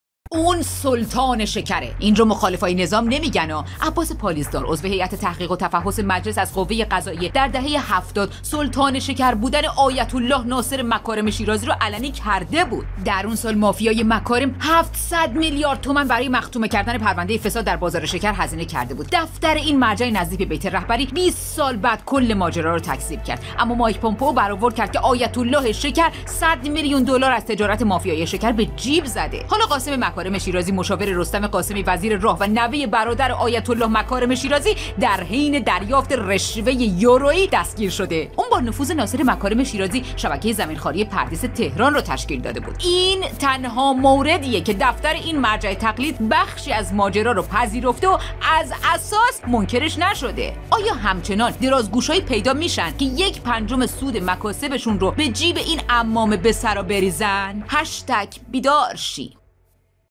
سلطان شکر و نوه‌ی فاسدش
آیت الله شکر و سلطان شکر رو که دیگه همه می‌شناسیم. حالا نوه هم داره تمرین می‌کنه.
سلطان زمین خواری!